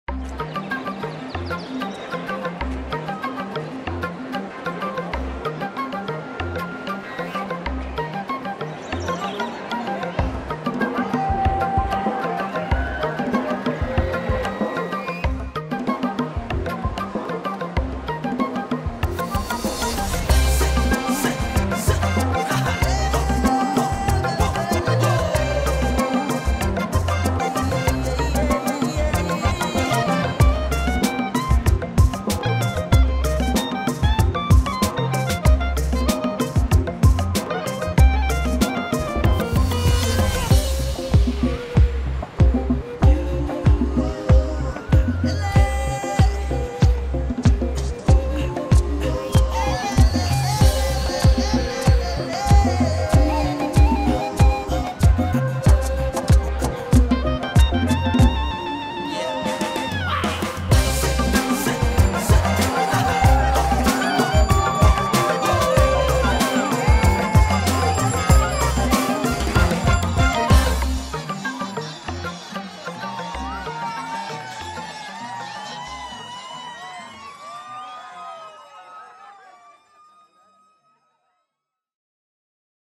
360º Feria de Las Flores 2017
Descubre algunos de los lugares que han hecho única esta FeriaDeLasFlores2017 y siéntete orgulloso de ser colombiano 🇨🇴😎